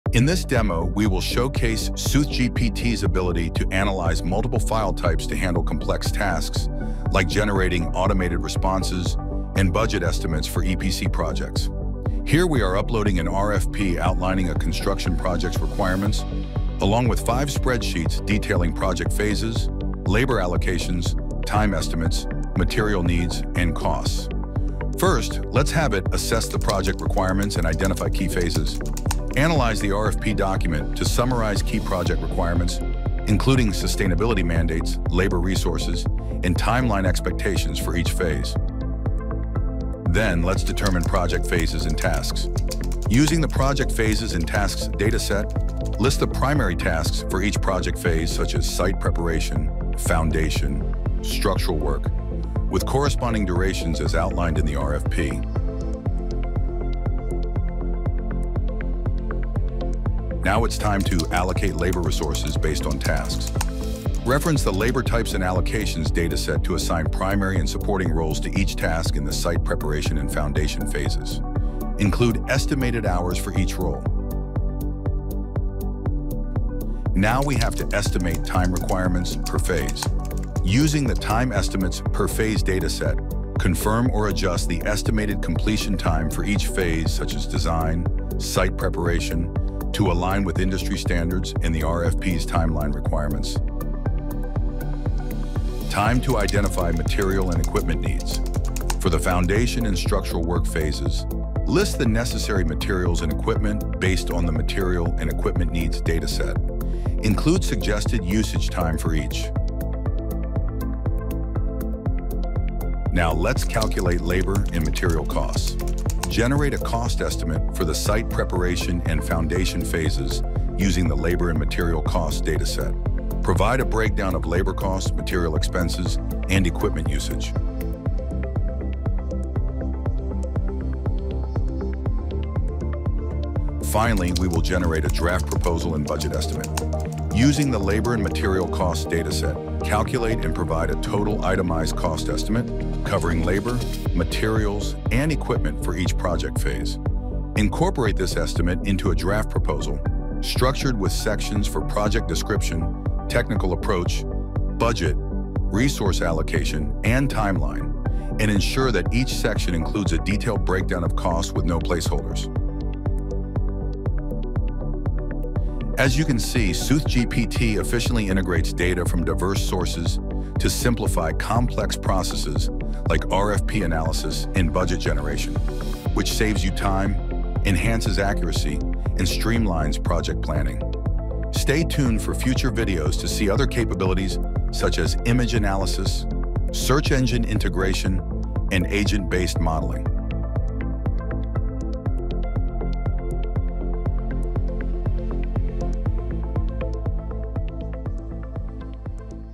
GenAI-Driven RFP Response & Budget Estimate Generation | SoothGPT | Soothsayer Analytics
Discover how SoothGPT simplifies RFP Analysis & Budget Generation by integrating data from multiple sources.